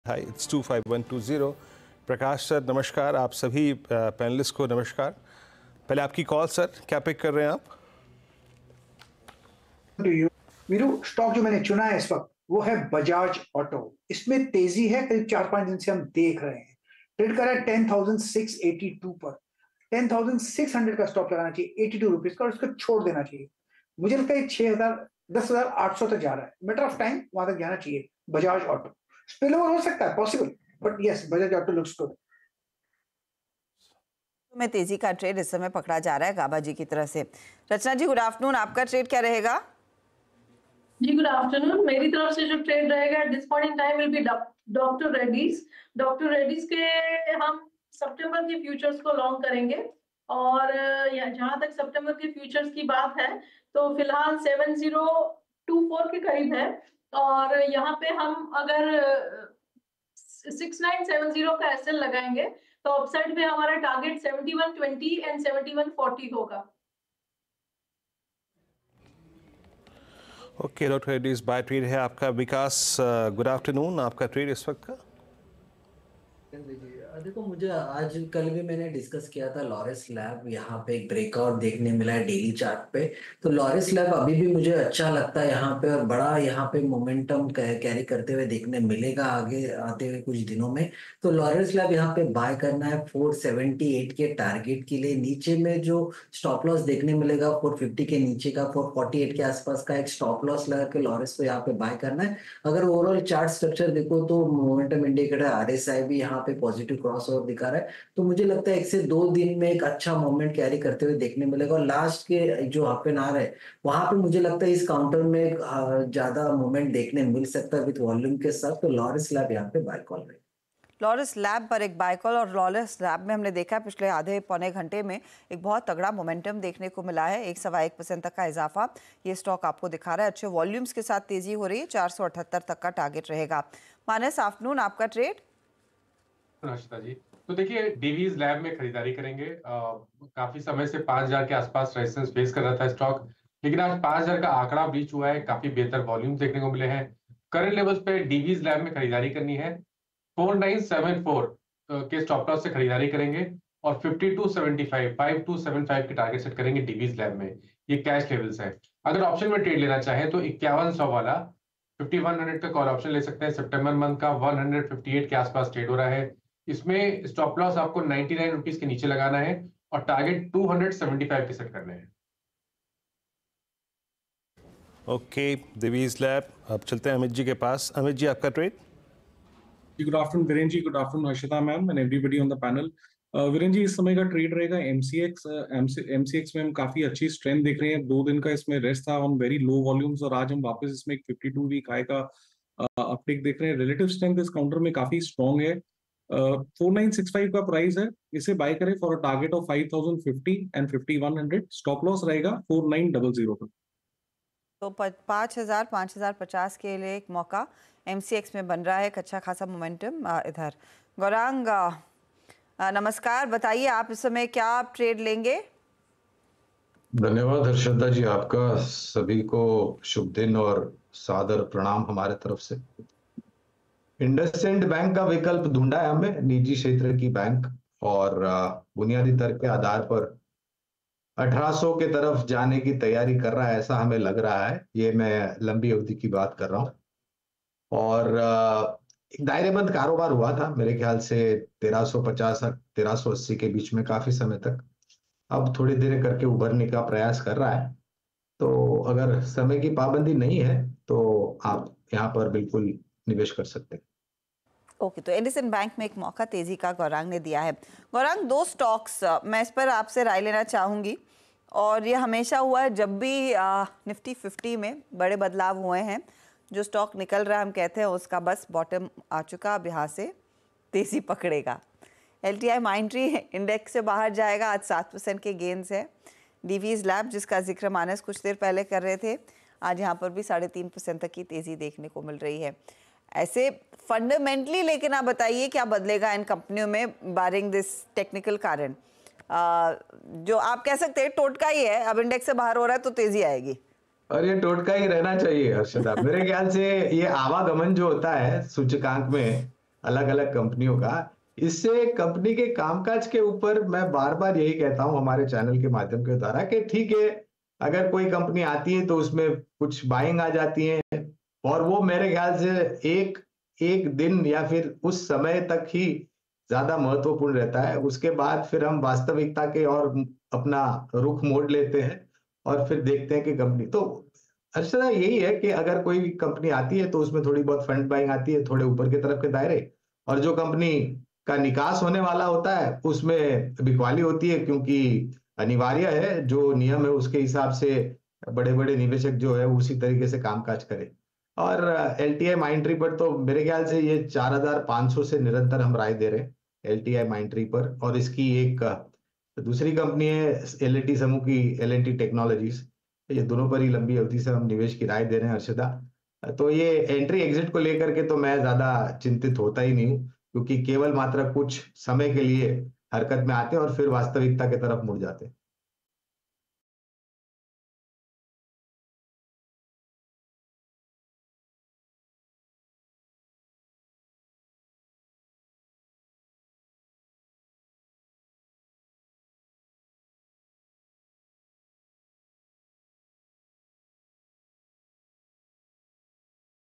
Top Trades For Tomorrow: बाजार खुलने पर इन Stocks में दिखेगा Action, आज ही तैयार करें निवेश
Top Trades में जानें कैसा है मार्केट का मूड महौल. Experts कल के लिए कहां निवेश करने की दे रहें है सलाह. देखें वीडियो.

n18oc_business